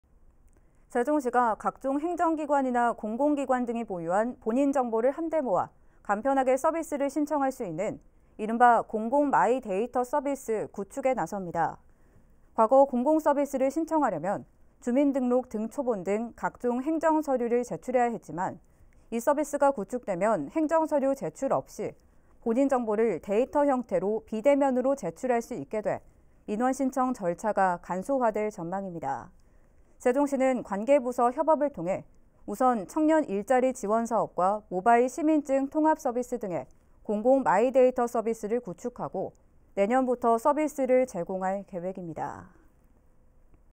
[B tv 세종뉴스]세종시, 본인 정보 간편 제출 '공공마이데이터 서비스' 구축
세종시가 각종 행정기관이나 공공기관 등이 보유한
본인 정보를 한데 모아 간편하게 서비스를 신청할 수 있는
이른바 ‘공공마이데이터 서비스’ 구축에 나섭니다.

과거 공공서비스를 신청하려면
주민등록등초본 등 각종 행정서류를 제출해야 했지만,
이 서비스가 구축되면 행정서류 제출 없이
본인정보를 데이터 형태로 비대면으로 제출할 수 있게 돼
민원신청 절차가 간소화 될 전망입니다.

세종시는 관계부서 협업을 통해
우선 청년 일자리 지원사업과 모바일 시민증 통합서비스 등에
공공마이데이터 서비스를 구축하고,
내년부터 서비스를 제공할 계획입니다.